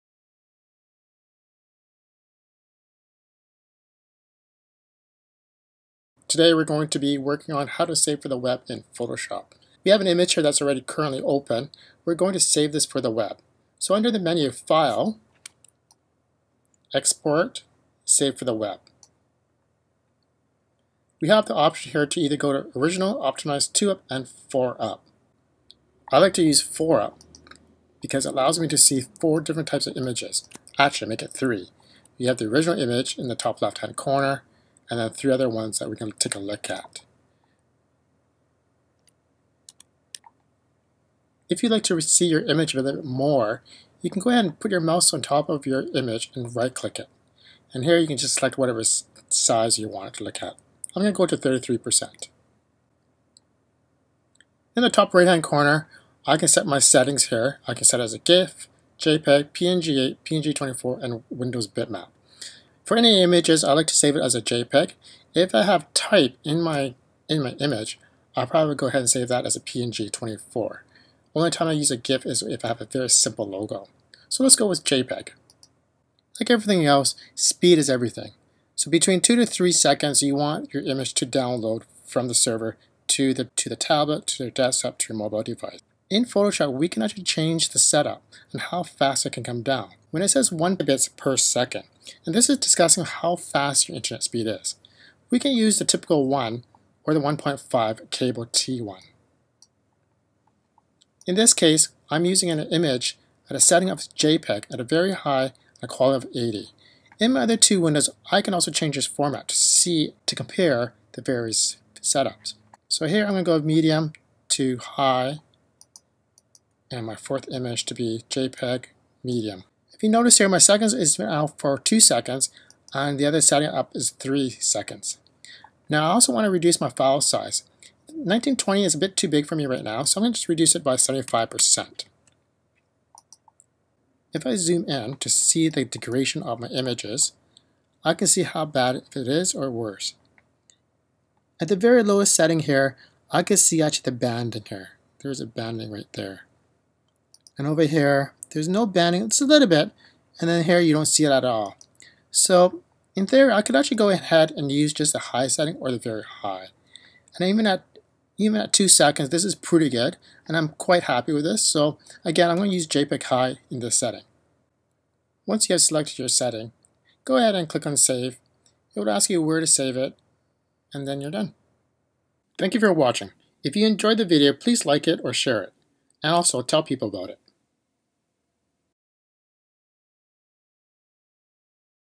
How to Use Saving for the Web in Photoshop
Placing images on the web does matter for everyone. Users who are using your website may leave if the images don't download within a reasonable time frame. Typically, that can be 2-3 seconds. Using Photoshop, Save for the Web makes it that easy for you to see how it compares to the original image and three other file formats. Yes, size does matter!

And let me know your thoughts and comments too!

ulearn.tech